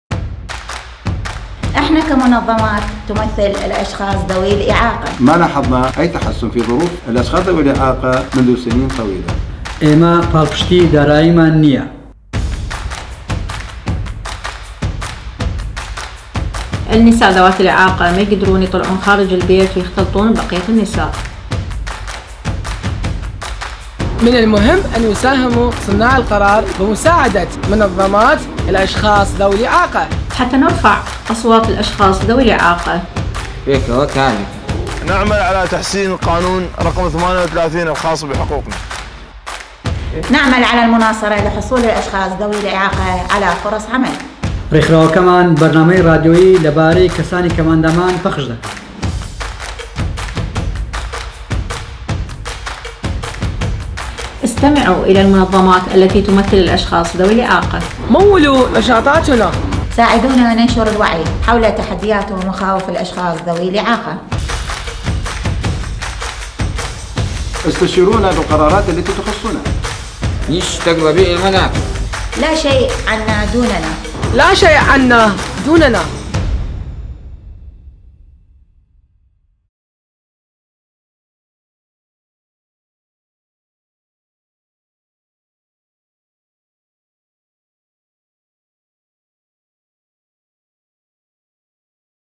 هیچ شتێک بێ ئێمە لەبارەی ئێمەوە مەکەن - ڕێکخراوەکانی کەسانی خاوەن پێداویستی تایبەت لە عێراق
عێراق خاوەنی یەک لە گەورەترین کۆمەڵگەکانی کەسانی خاوەن پێداویستیی تایبەتە لە جیهاندا، بەڵام حکوومەت و ڕێکخراوە مرۆییەکان و ڕێکخراوەکانی تایبەت بە پەرەپێدان ڕاوێژی کەمیان کردووە بە کەسانی خاوەن پێداویستی تایبەت و ئەو ڕێکخراوانەی نوێنەرایەتییان دەکەن. ڕێکخراوی نێودەوڵەتی کۆچ دیمانەی لەگەڵ ٨١ نوێنەری ٥٣ ڕێکخراوی تایبەت بە کەسانی خاوەن پێداویستی تایبەت لە ١٨ پارێزگای عێراق ئەنجام داوە – بۆ باشترکردنی تێگەیشتن لە بەربەستەکانی بەردەم کەسانی خاوەن پێداویستی تایبەت، هەروەها بۆ باشتر ڕێنوێنیکردنی بەرنامەڕێژی مرۆیی و پەرەپێدان. ئەم ڤیدیۆیە هەندێک لە سەرنجەکانی ئەوان دەخاتە ڕوو.
بە پاڵپشتی دارایی وەزارەتی دەرەوە و بازرگانیی حکوومەتی ئوسترالیا